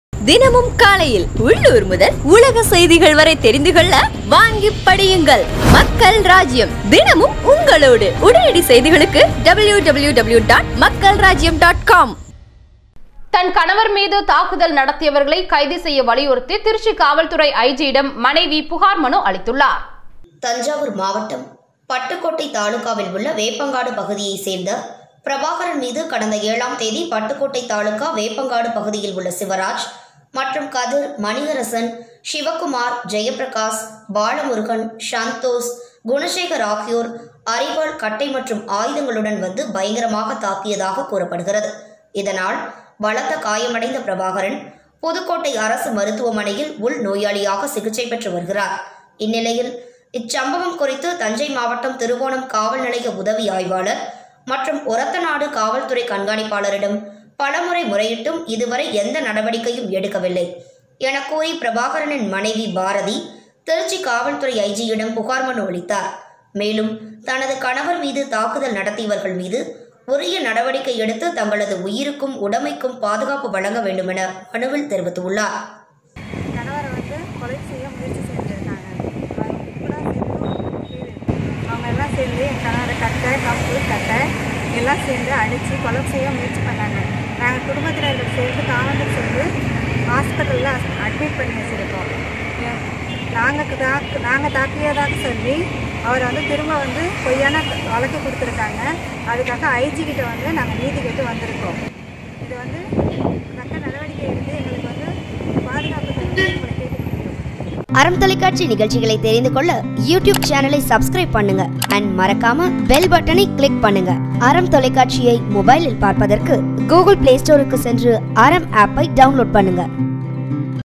கணவர் மீது தாக்குதல் நடத்தியவர்களை கைது செய்ய வலியுறுத்தி மனைவி புகார்
தன் கணவர் மீது தாக்குதல் நடத்தியவர்களை கைது செய்ய வலியுறுத்தி திருச்சி காவல்துறை ஐஜியிடம் மனைவி புகார் மனு அளித்துள்ளார்